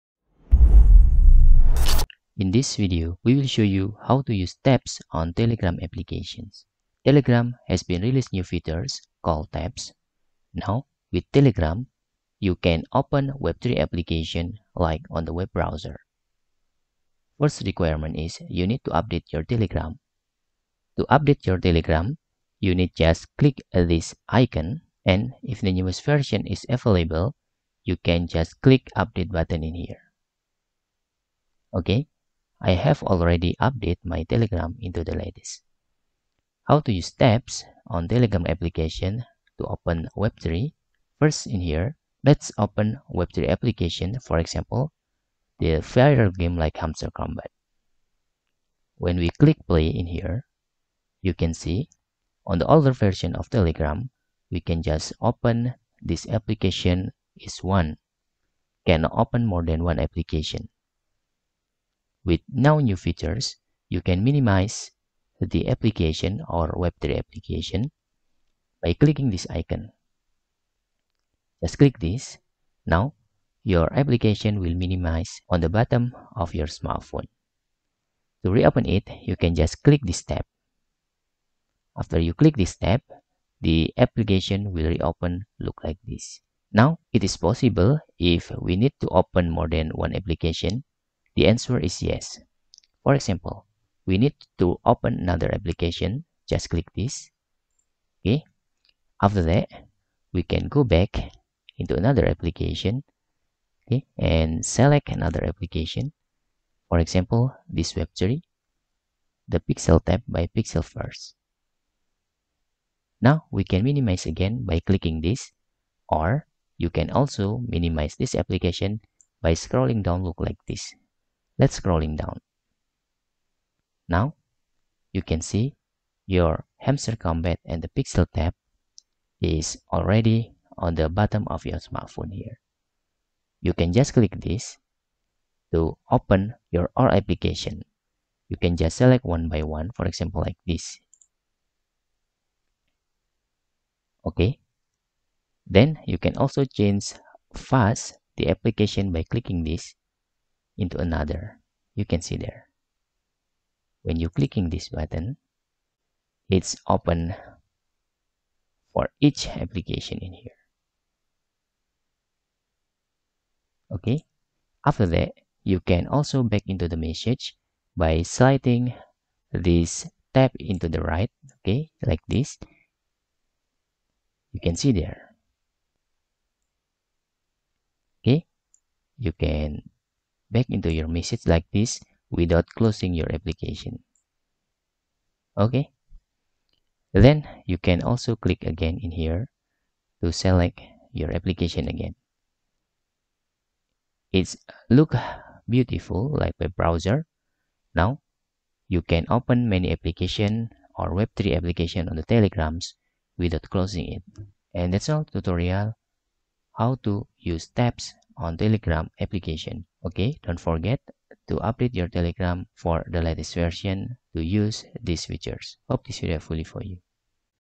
How to Use Tabs on Telegram apps
Now with the Tab feature in Telegram, you no longer need to close web3 applications if you want to open other applications or even open messages.